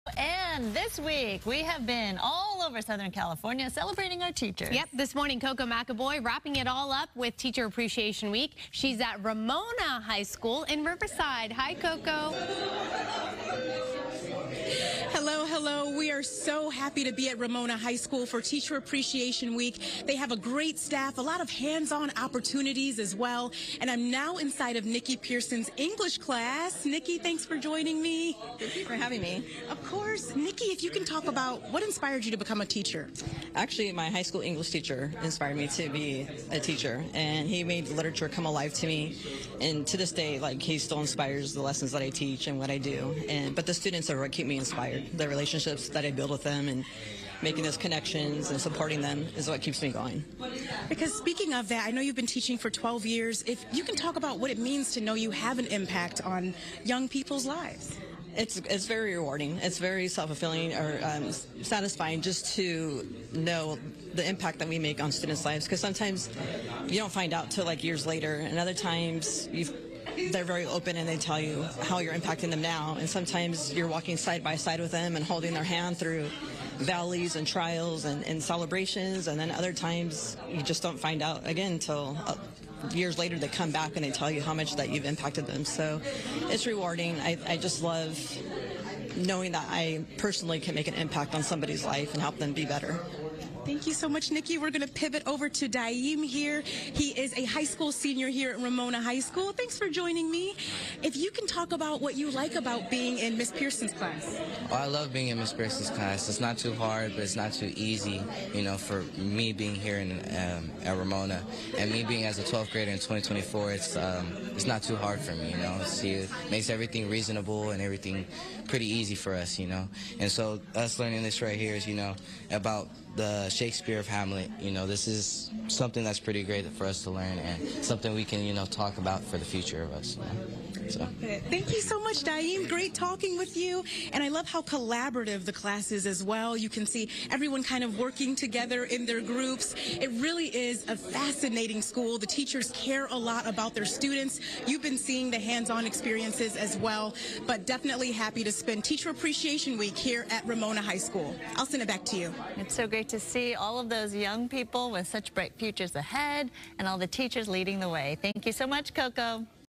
Teacher Appreciation Week at Ramona High School in Riverside
FOX 11's Koco McAboy takes us to Ramona High School in Riverside.